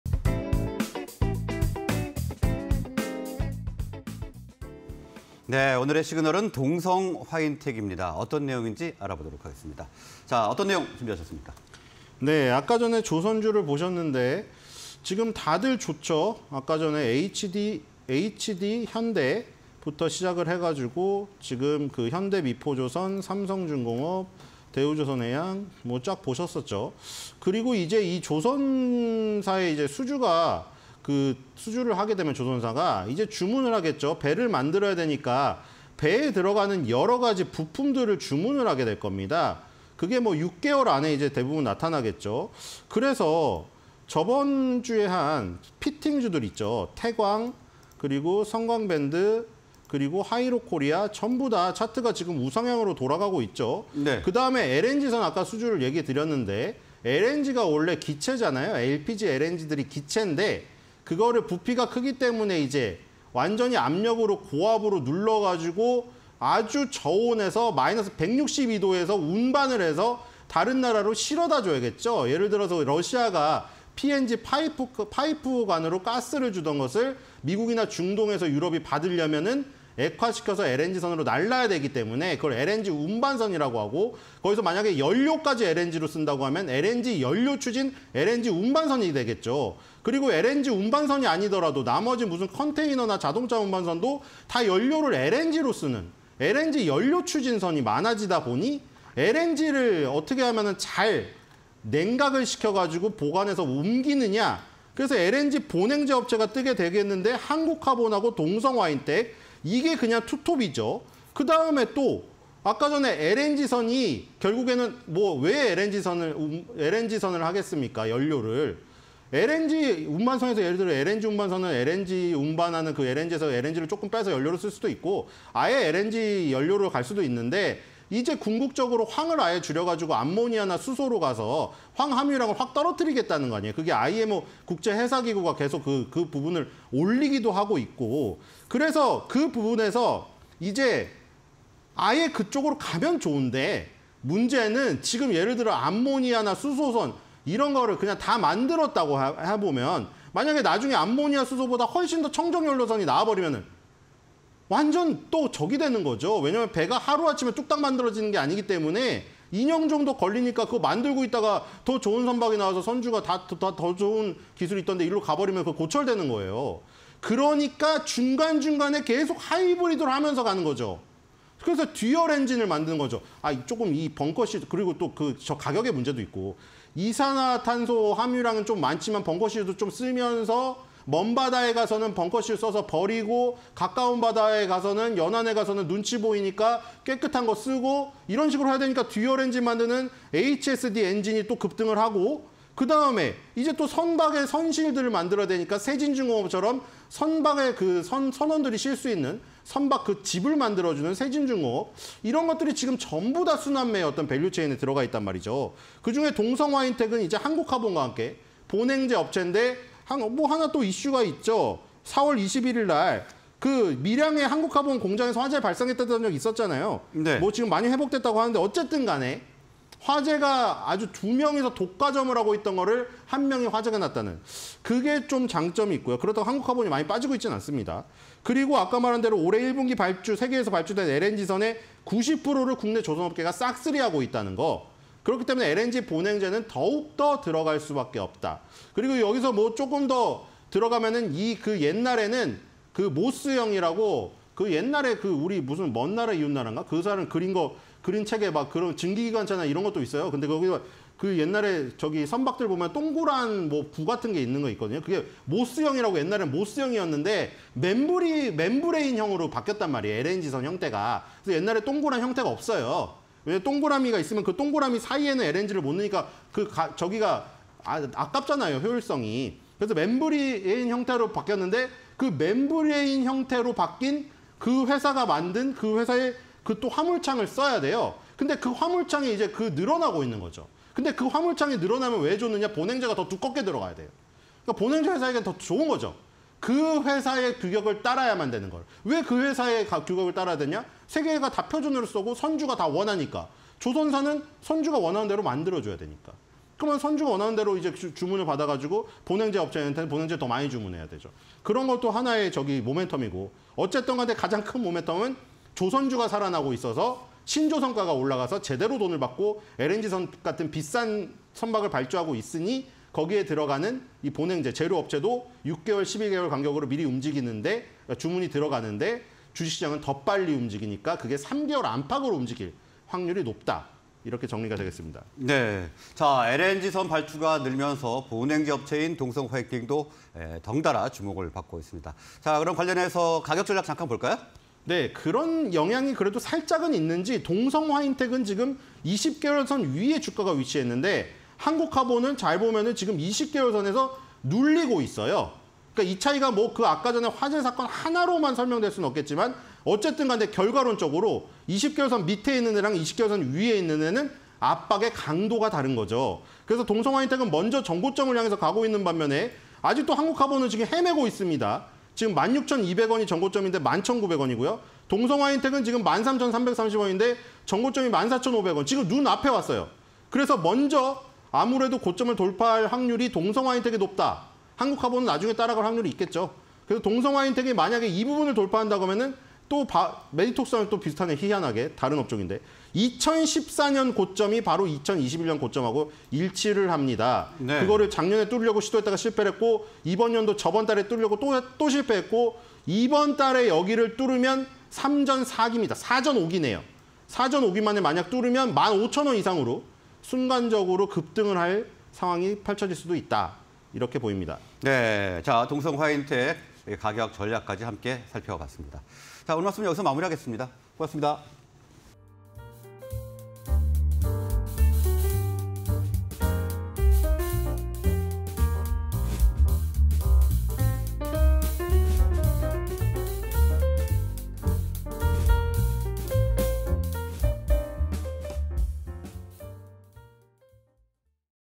동성화인텍(033500), 주가 레벨업… "LNG선 발주의 후광"_신소장의 시그널 (20230710)
동성화인텍(033500) "LNG선 발주의 후광"
- LNG(액화천연가스) 보냉재 제조
- 국내 동성화인텍·한국카본 시장 과점
- 한국카본 반사익… 잇따른 대규모 수주